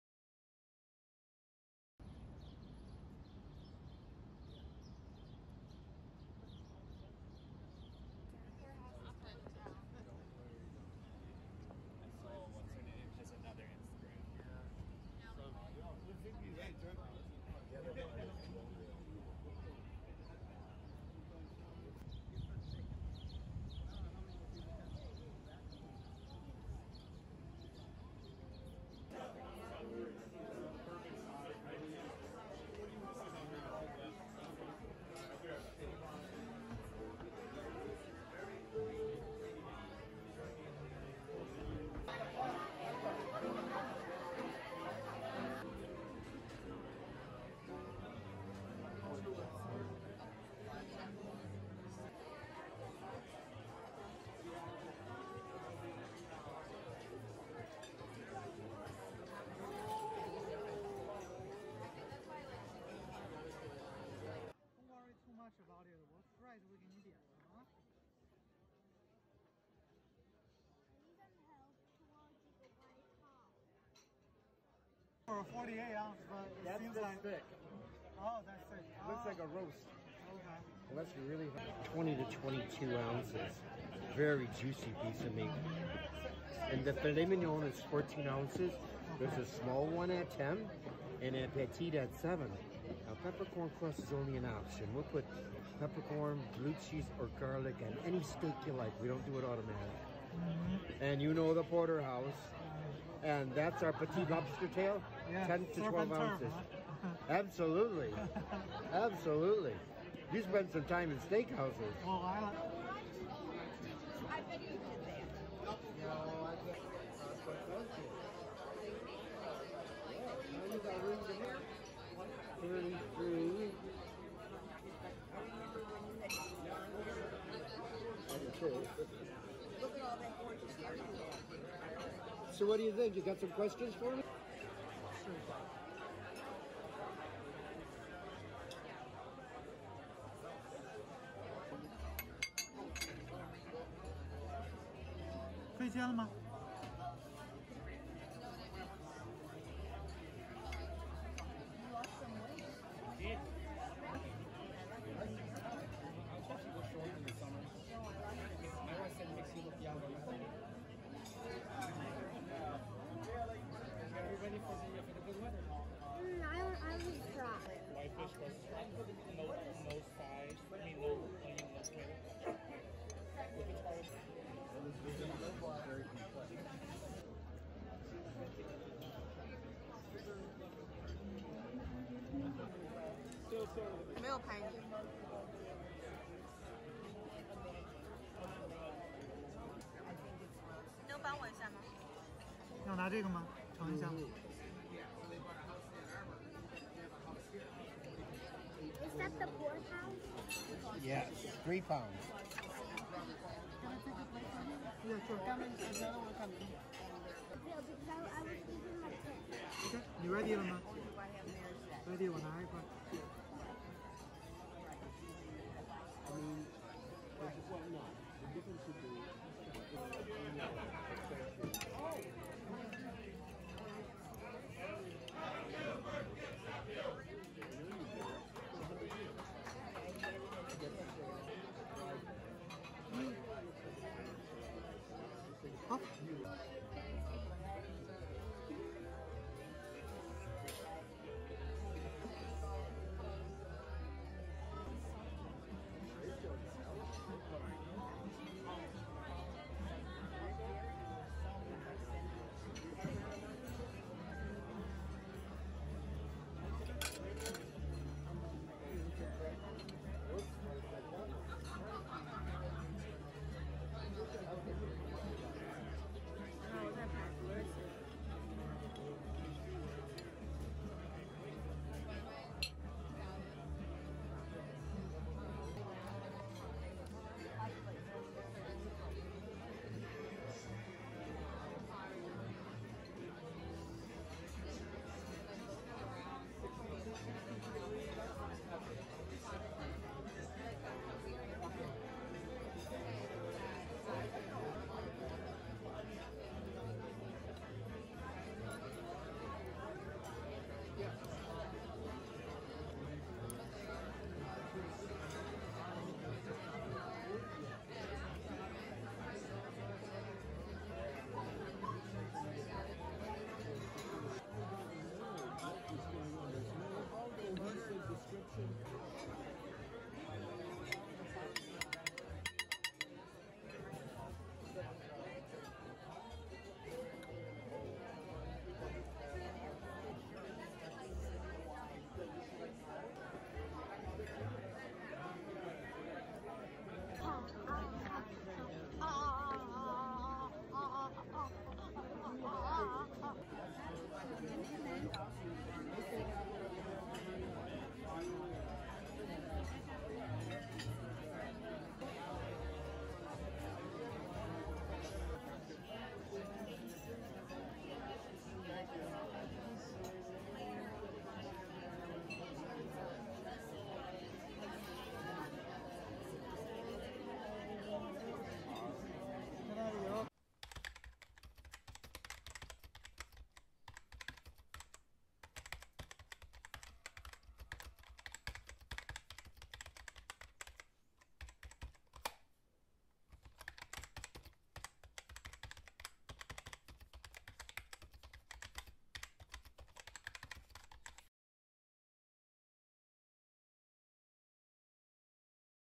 Chicago Iconic Steakhouse | Gibsons Bar & Steakhouse | $150
PorterhouseWe are sharing our experience in the Gibsons Bar & Steakhouse in downtown Chicago. It's a Chicago icon, with great steak and seafood. Let's check it out together!